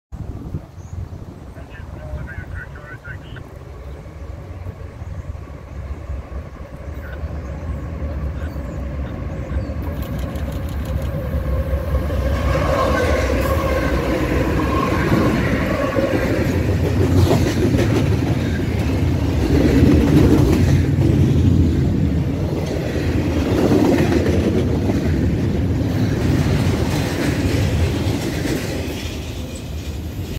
CPR 8173 at Howland (AUG 26/22)
CPR 8173, CPR 8143, CPR 8918 (CPR 101) (11 cars) races by Howland as they begin the journey towards Vancouver.

MP 3.5 CPR North Toronto sub
Toronto, Ontario
1646h